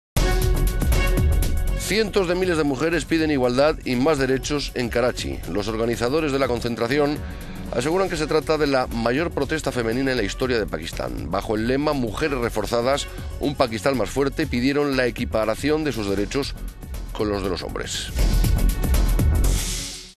Histórica manifestación de mujeres en Karachi por la igualdad
Cientos de miles de mujeres piden igualdad y más derechos en Karachi. Los organizadores de la concentración aseguran que se trata de la mayor concentración femenina en la historia de Pakistán. Bajo el lema "mujeres reforzadas, un Pakistán más fuerte", pidieron la equiparación de sus derechos con los de los hombres.